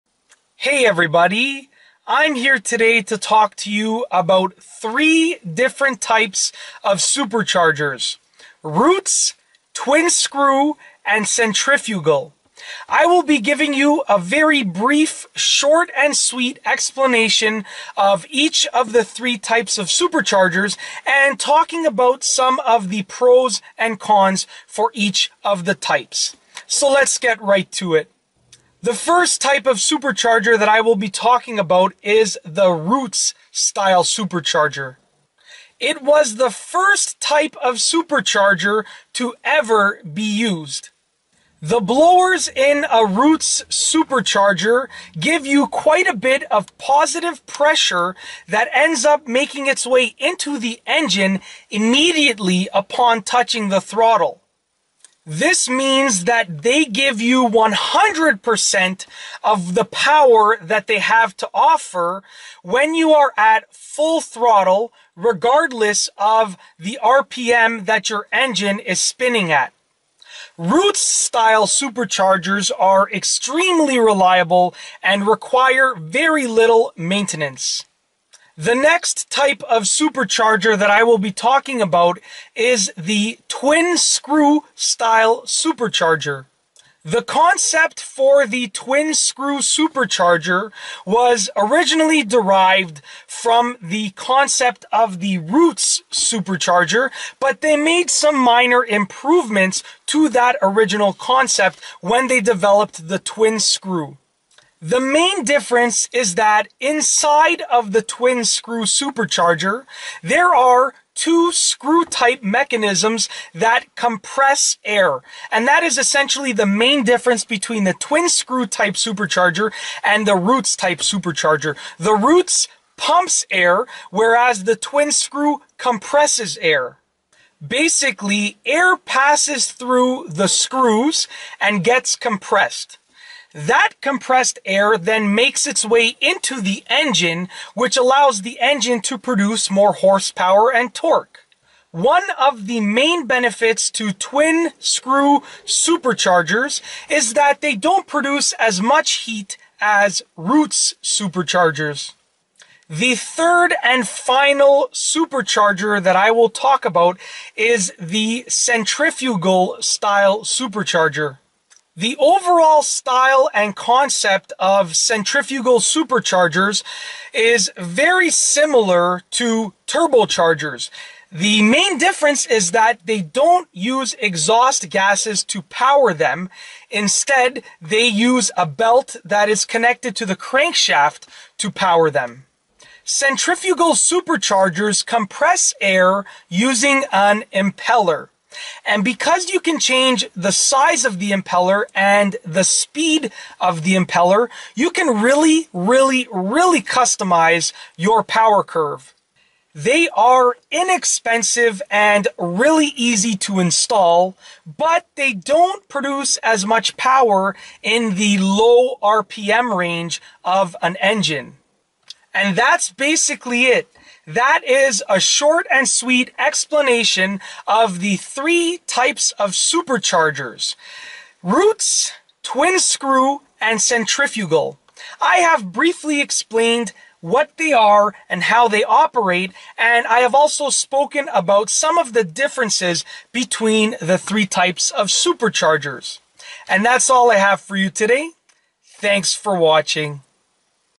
Superchargers-Roots VS Twin Screw VS Centrifugal (Forced Induction)
In this video, I talk about 3 different types of superchargers: roots, twin screw, and centrifugal. There are some similarities and differences between these 3 types of superchargers. I briefly explain how they work, and talk about some of the similarities and differences.